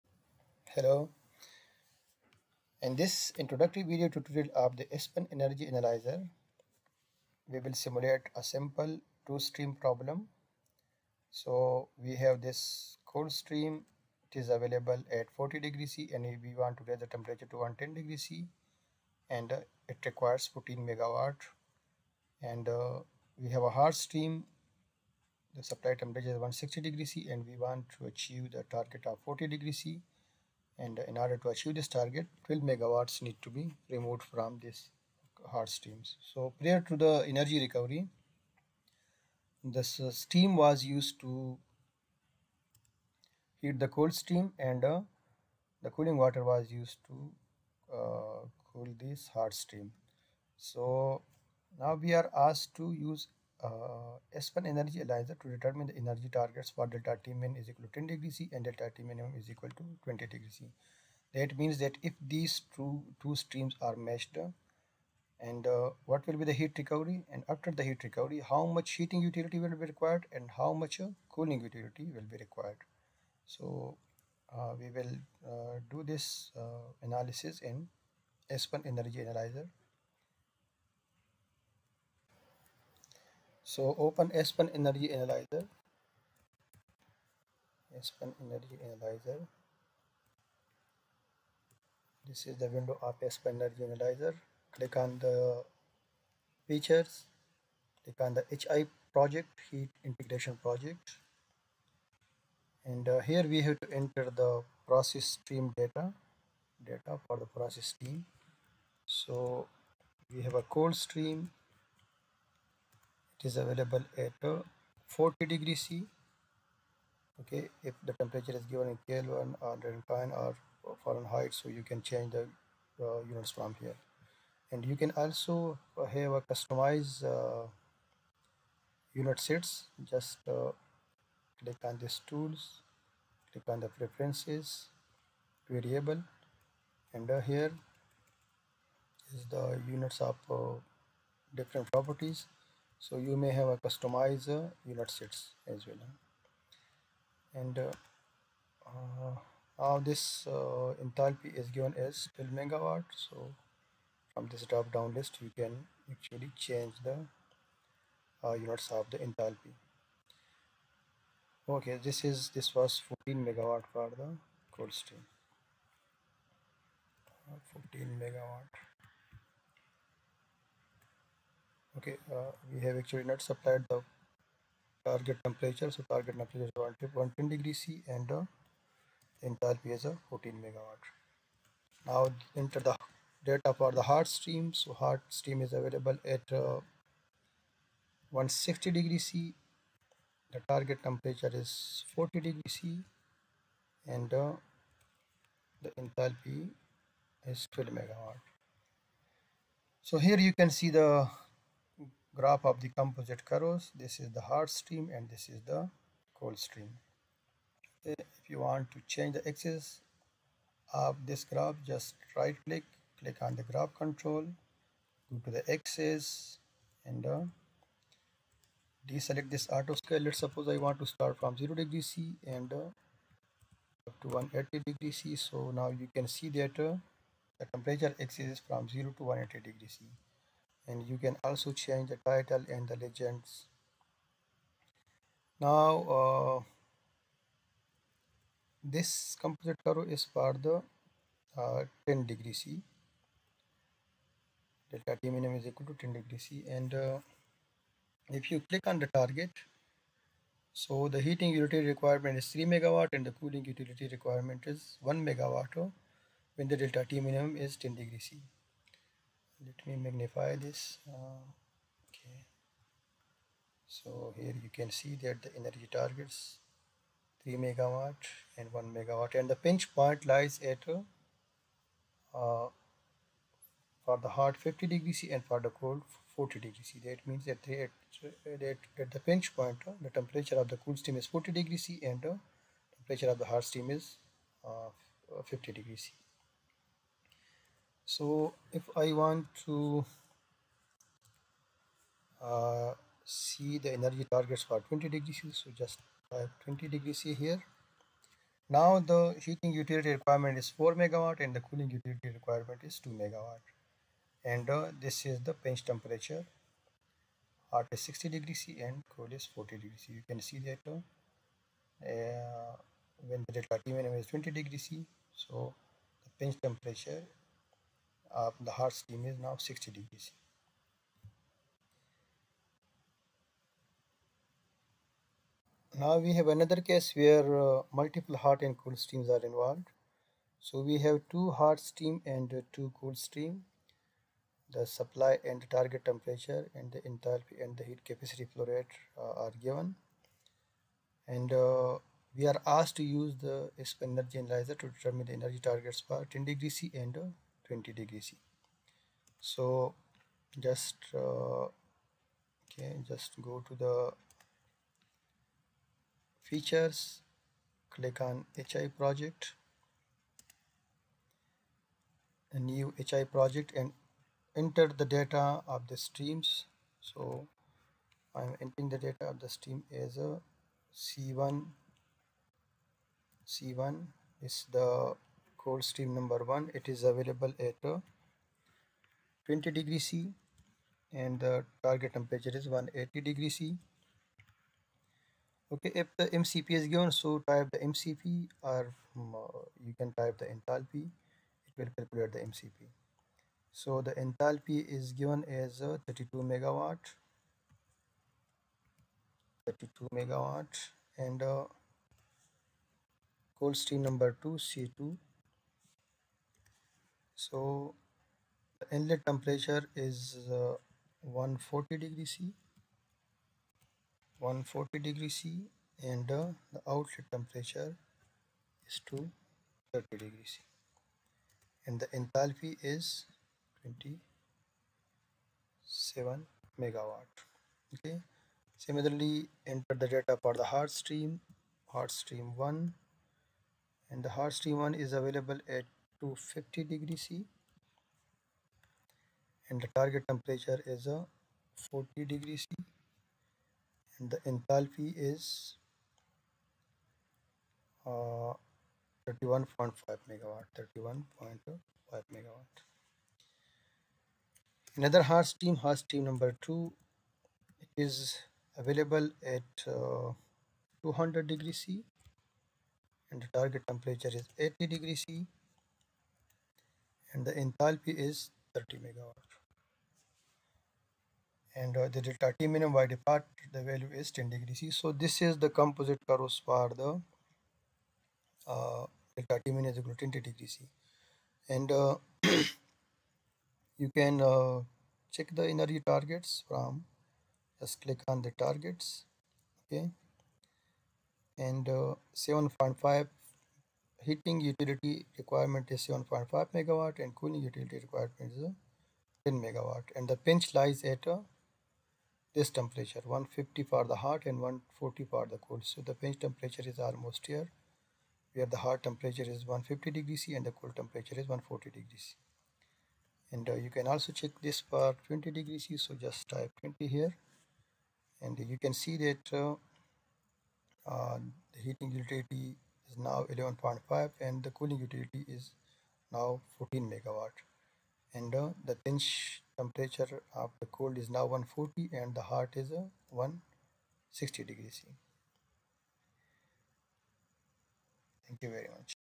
Getting Started with ASPEN Energy Analyzer-Part 1: Compsite Curves and Energy Targets
Here we plot the composite curves and determine the energy targets for two cases using ASPEN Energy Analyzer. The first case involves single hot and cold streams. In the second case, we determine the targets for mutiple hot and cold streams.